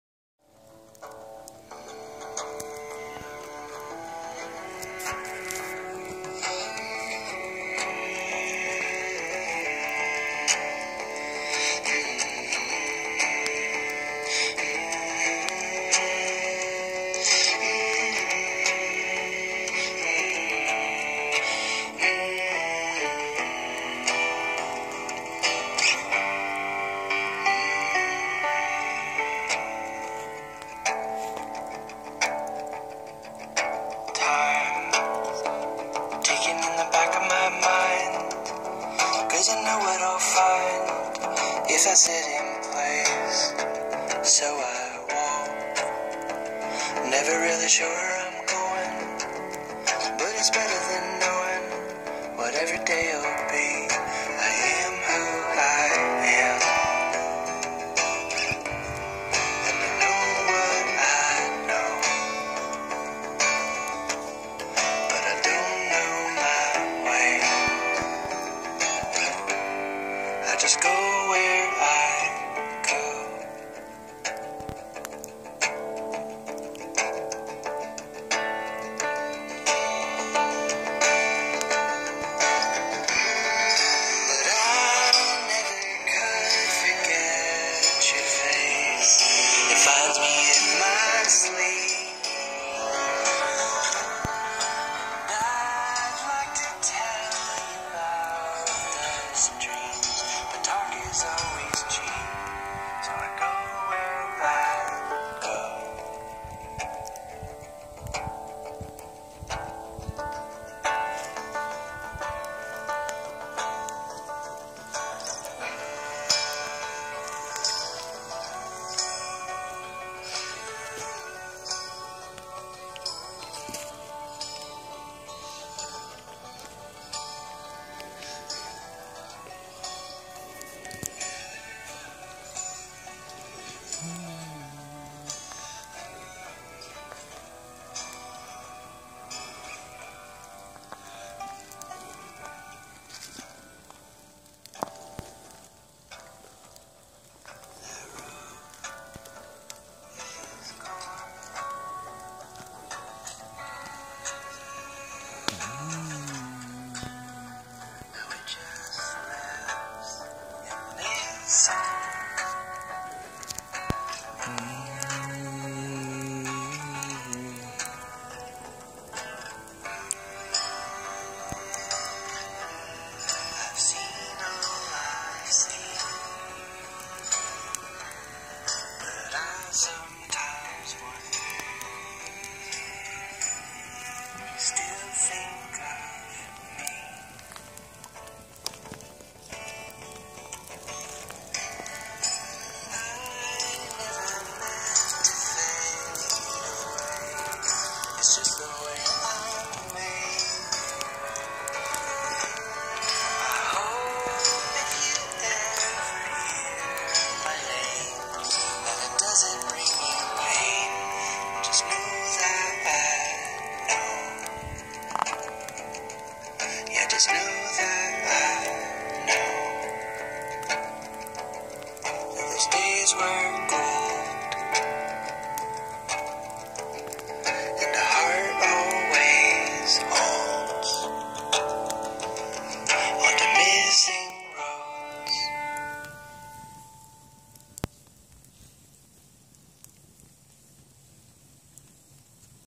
Witching Hour Fire with TheHauntMaster and Radical Face Oct 01 2020
A Witching Hour Fire with The HauntMaster and Radical Face from a cellphone Oct 01 2020